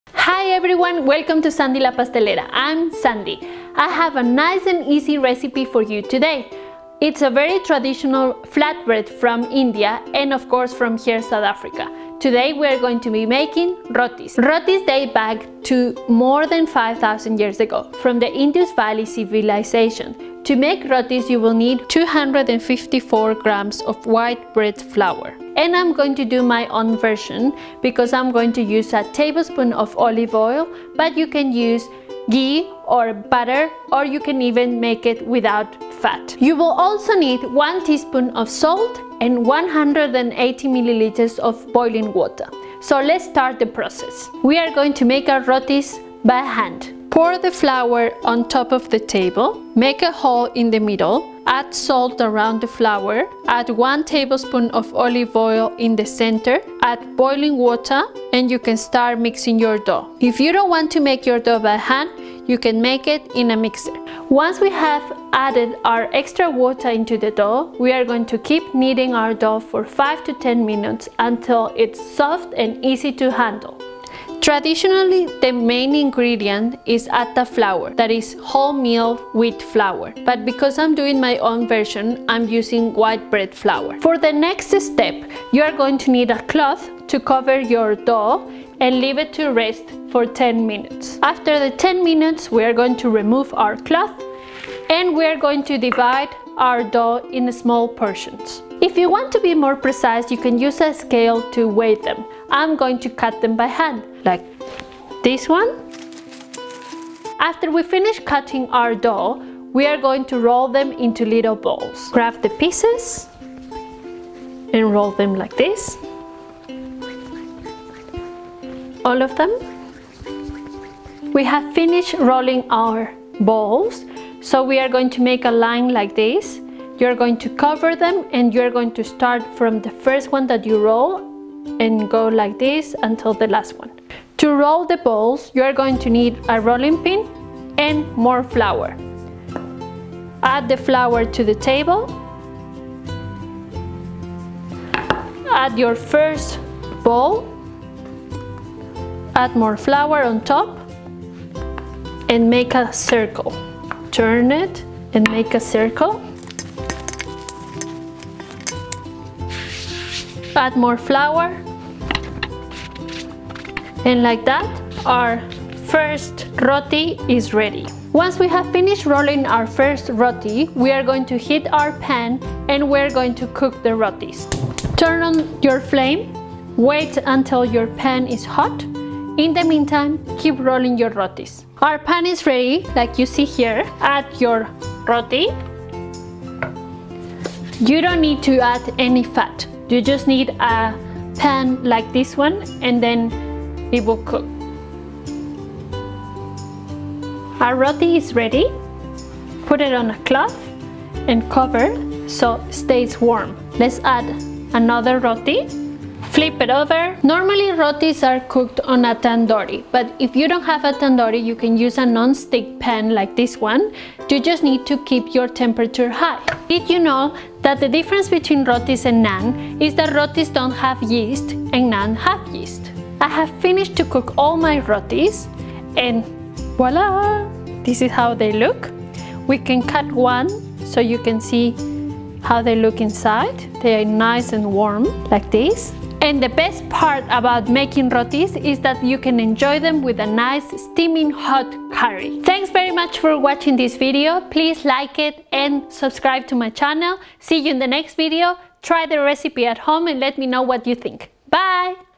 Roti Recipe
Rotis are very traditional flat bread from India. They are very well-known all over the world. In this video I am going to teach you how to make Rotis in my own way. Using white bread flour and olive oil.

Visita Sandy la Pastelera para leer más recetas y ver más videos.

Los Rotis son panes planos muy tradicionales de India. Son conocidos en muchas partes del mundo y lo hacen de diferentes maneras. En este video te voy a enseñar a realizar Rotis a mi propio estilo. Usando harina de trigo - Harina fuerte con aceite de oliva. Si tienes alguna duda sobre el video por favor solo ponlo en los comentarios y con gusto te responderé.